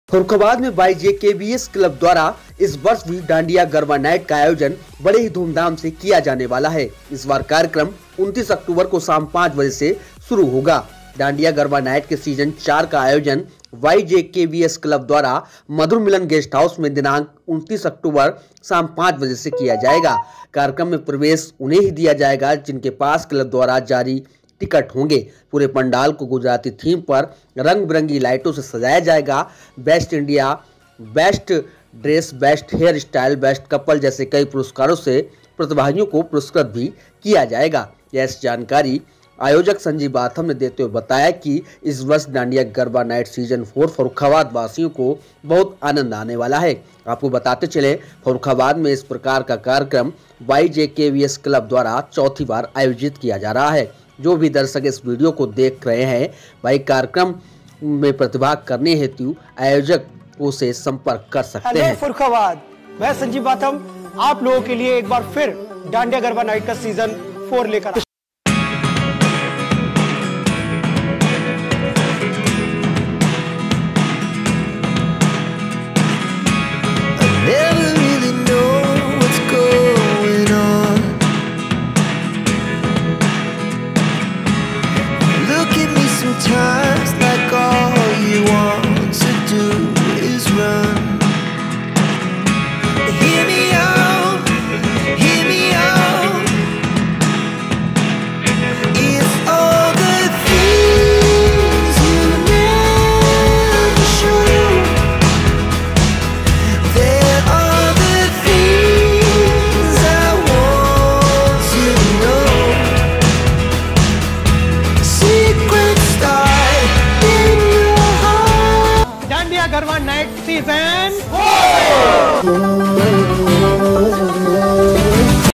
फर्रुखाबाद : डांडिया गरबा नाइट सीजन 4 का आयोजन 29 अक्टूबर को YJKBS क्लब द्वारा होगा आयोजित।
डांडिया गरबा नाईट के सीजन 4 का आयोजन वाई जे के वी एस क्लब द्वारा मधुर मिलन गेस्ट हाउस मे दिनांक 29 अक्टूबर 2023 को शाम 05 बजे से किया जायेगा। कार्यक्रम में प्रवेश उन्हे ही दिया जायेगा जिसके पास क्लब द्वारा जारी टिकट होंगे। पूरे पांडाल को गुजरती थीम पर रंगबिरंगी लाइटों से  सजाया जायेगा। बेस्ट डांडिया, बेस्ट ड्रेस, बेस्ट हेयर स्टाइल, बेस्ट कपल जैसे कई पुरुस्कारों से प्रतिभागियों को पुरुस्कृत किया जायेगा।

डांडिया गरबा नाइट सीजन 4 का आयोजन 29 अक्टूबर को YJKBS क्लब द्वारा होगा आयोजित।